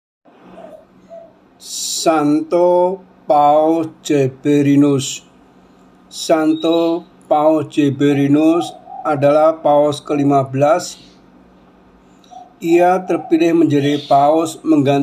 Short Kisah Santo Paus Zepherinus 20 Desember
SANTO  PAUS ZEPHERINUS

Paus ke-15

Santo Paus Zepherinus adalah paus  ke-15. Ia terpilih menjadi Paus menggantikan Paus Victor I pada tahun 199.  Paus Zepherinus memimpin Gereja dalam situasi yang sangat sulit.

Kaisar Lucius Septimus Severus mulai menganiaya umat Kristen. Banyak umat yang ditangkap dan dihukum mati. Zephirinus harus berjuang keras melindungi umat Allah yang sedang dianiaya.

Selain itu, Paus Zepherinus juga harus mempertahankan kebenaran ajaran iman dari Rongrongan para pengikut bidaah "Dynamic Monarchianism" atau "Adoptionism"  yang dicetuskan oleh Theodotus ( Theodotus the Leather seller )

Theodotus sebenarnya telah di-ekskomunikasi oleh  Paus Victor I,  namun ia dan para pengikutnya tetap berupaya untuk menyebarkan ajaran sesat